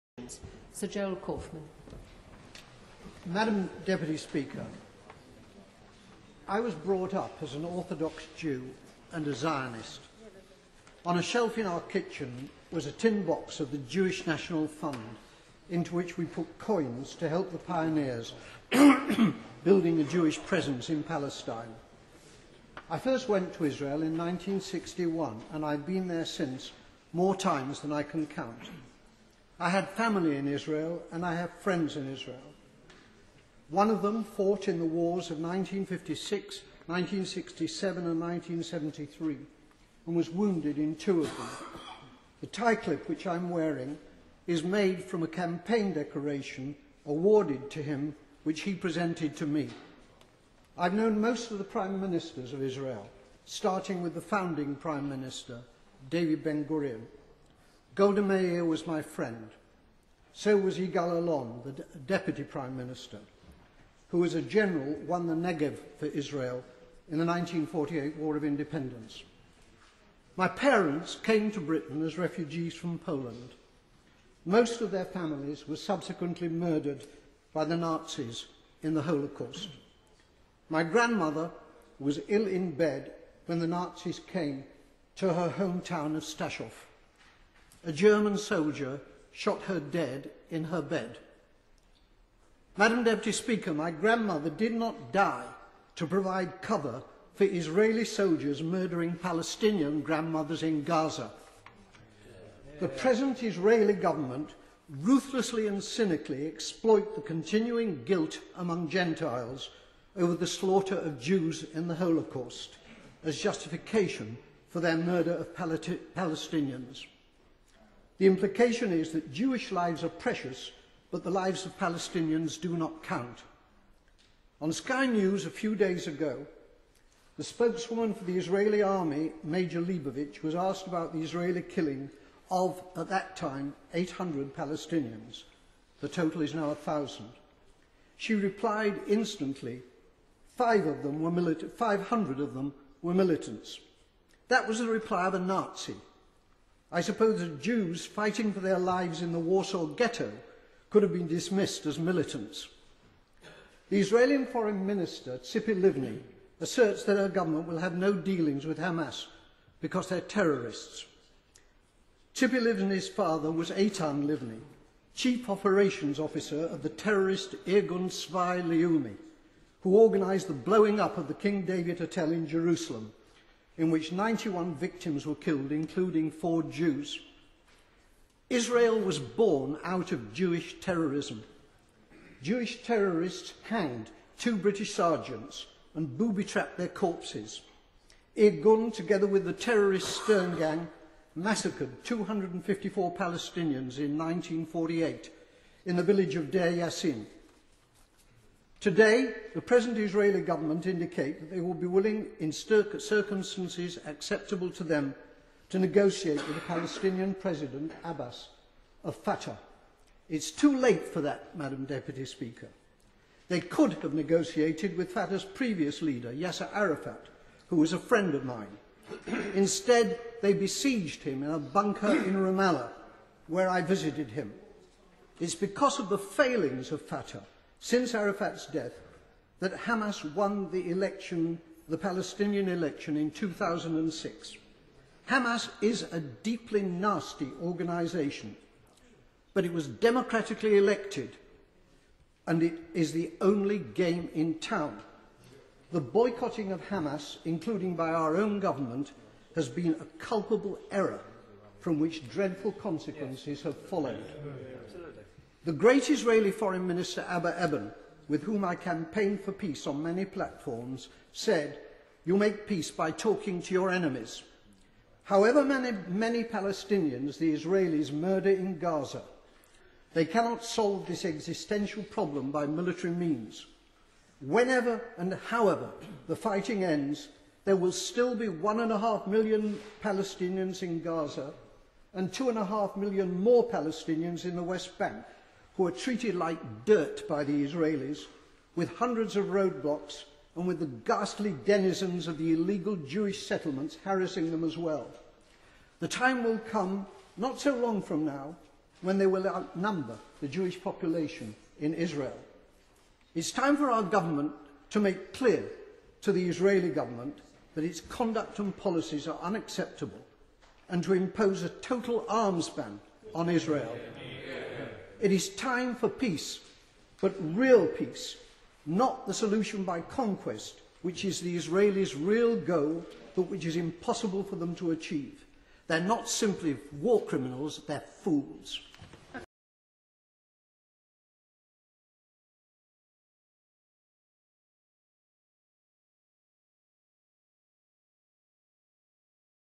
UK Jewish MP: Israel acting like Nazis in Gaza
"Israel was born out of Jewish Terrorism" Tzipi Livnis Father was a Terrorist" Astonishing claims in the Hous of Parliamnet. SIR Gerald Kaufman, the veteran Labour MP, yesterday compared the actions of Israeli troops in Gaza to the Nazis who forced his family to flee Poland.

During a Commons debate on the fighting in Gaza, he urged the government to impose an arms embargo on Israel.

Sir Gerald, who was brought up as an orthodox Jew and Zionist, said: "My grandmother was ill in bed when the Nazis came to her home town a German soldier shot her dead in her bed.

"My grandmother did not die to provide cover for Israeli soldiers murdering Palestinian grandmothers in Gaza. The present Israeli government ruthlessly and cynically exploits the continuing guilt from gentiles over the slaughter of Jews in the Holocaust as justification for their murder of Palestinians."

He said the claim that many of the Palestinian victims were militants "was the reply of the Nazi" and added: "I suppose the Jews fighting for their lives in the Warsaw ghetto could have been dismissed as militants."

He accused the Israeli government of seeking "conquest" and added: "

They are not simply war criminals, they are fools."